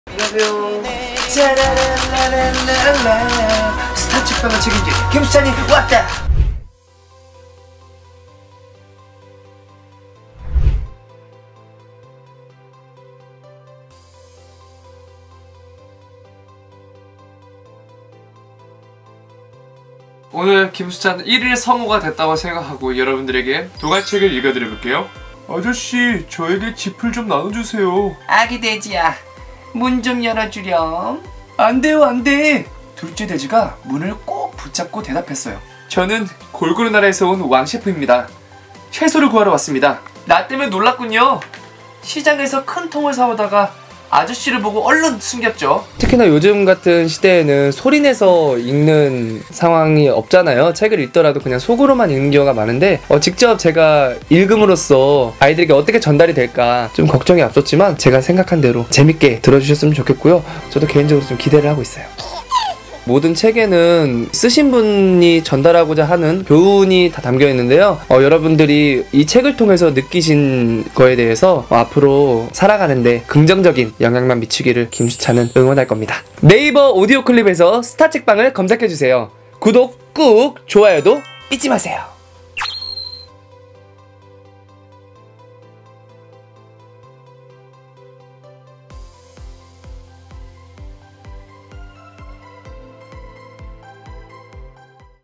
[텐아시아 스타책방] 릴레이 재능기부 - 김수찬 편 "긍정적인 영향 주고 싶어요"
트로트 가수 김수찬이 텐아시아와 네이버 오디오 클립이 주최한 '스타책방'에 다섯 번째 주자로 참여했다. '스타책방'은 스타들의 릴레이 재능기부 프로젝트로 청각장애 부모를 둔 아이들, 다문화 가정의 아이들에게 동화책을 읽어주는 취지로 제작됐다.

김수찬은 그레이트북스 안녕 마음아의 '골고루 나라의 왕셰프'와 그레이트북스 행복한 명작의 '아기 돼지 삼형제'를 읽었다. 그는 부드러운 목소리로 책 속의 다양한 역할을 섬세하게 그려내는 등 완성도 높은 리딩을 펼쳤다. 이어 유쾌한 매력으로 현장 분위기를 훈훈하게 물들였다.

녹음을 마친 후 김수찬은 "요즘 같은 시대에는 책을 소리 내서 읽을 일이 별로 없다. (책을) 읽더라도 속으로만 읽는 경우가 많다"면서 "내가 읽은 책을 아이들이 어떻게 받아들일까 싶어 걱정이 앞섰다. 재밌게 들어줬으면 좋겠고 나도 기대하고 있다"고 밝혔다.

그러면서 "모든 책에는 쓴 사람이 전달하고자 하는 교훈이 담겨 있다. 이 책을 통해 앞으로 살아가는 데 긍정적인 영향만 미치기를 응원한다"며 웃었다.

'스타책방'은 매주 월, 목요일 네이버 오디오 클립을 통해 공개되며 네이버 부모i 판에서도 만날 수 있다.